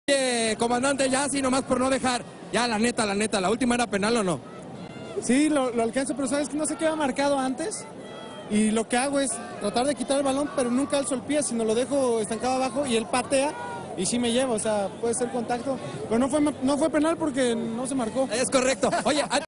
Jair Pereira reconoce que cometió penal sobre Ismael Sosa
El defensa de Chivas comentó la polémica jugada que pudo haber cambiado la final del Clausura 2017 y completó  diciendo “no fue penal porque no se marcó”.